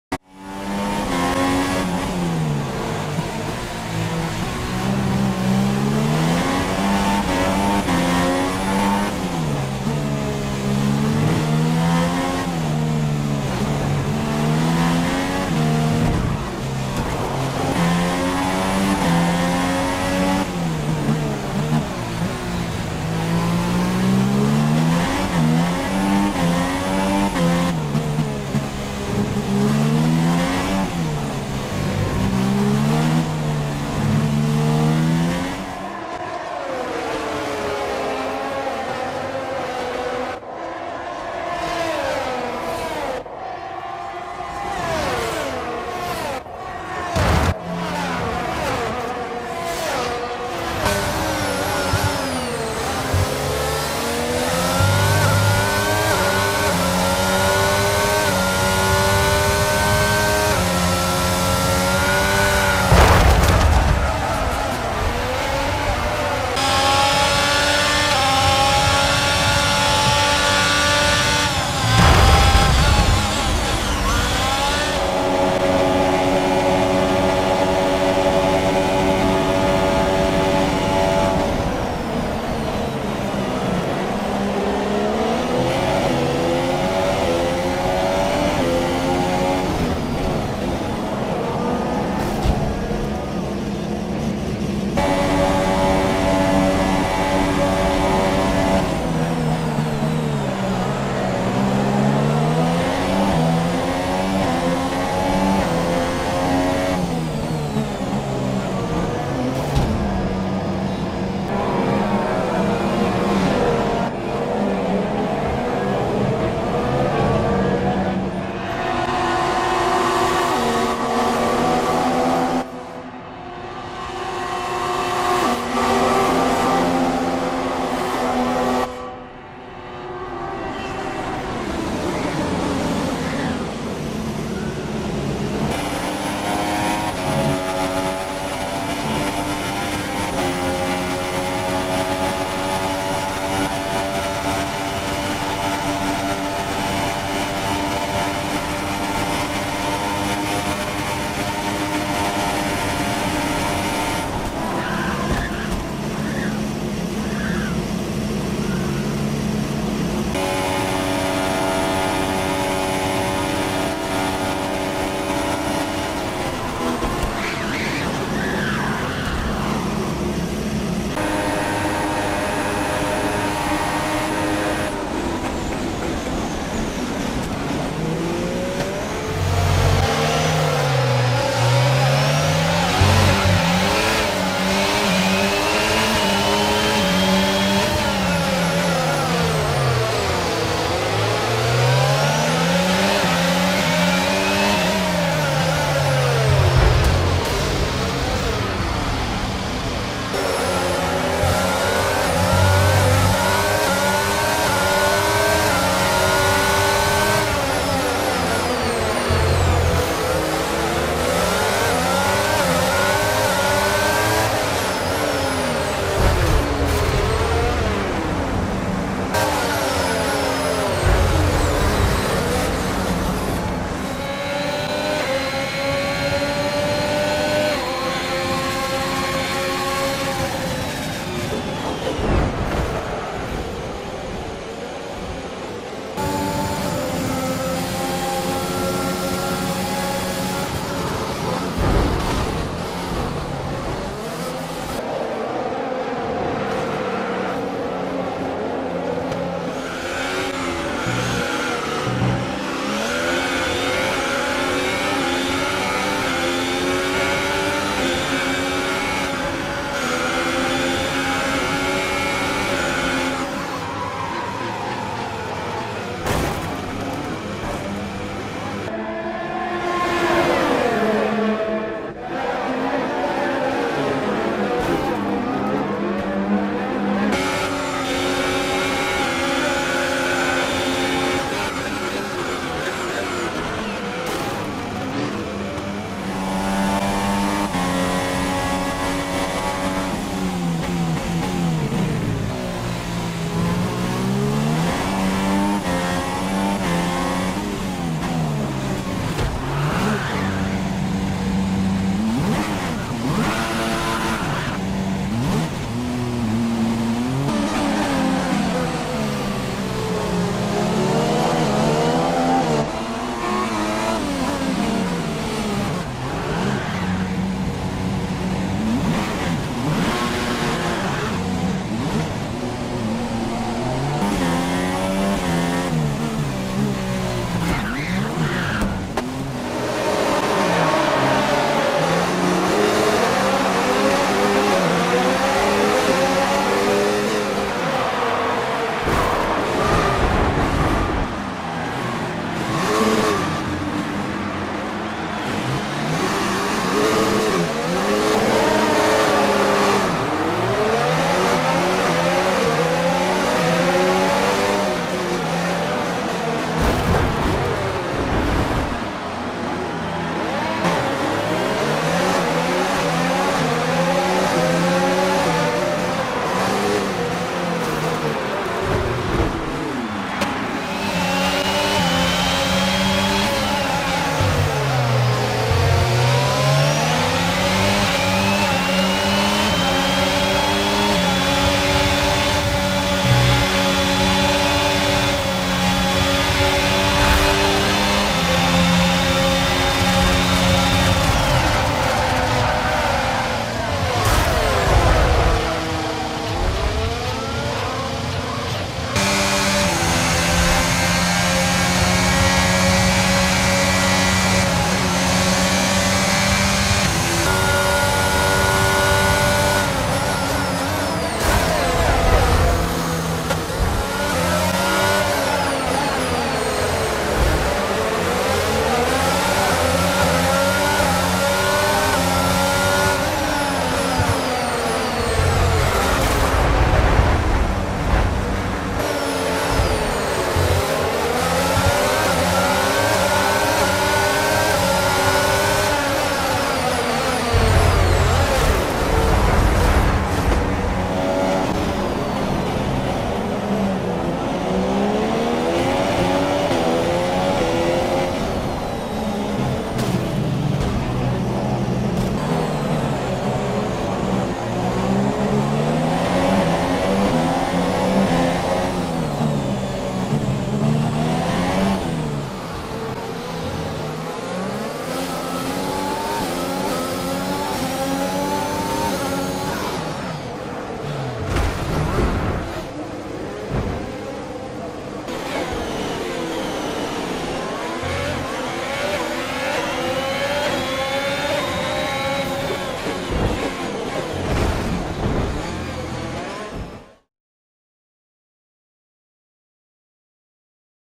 REALISTIC F1 CRASHES BAKU!
Hello and welcome to another realistic crashes episode from the current GP track on the F1 2023 calender. For the for the fourth round of the 2023 season we are finding ourselves in the small streets of Baku for the Azerbaijan GP! In this series we will have a look at realistic crashes that could have happened on this track. So make sure to sit back, relax and enjoy some realistic crashes from the Baku City Circuit for round four in 2023!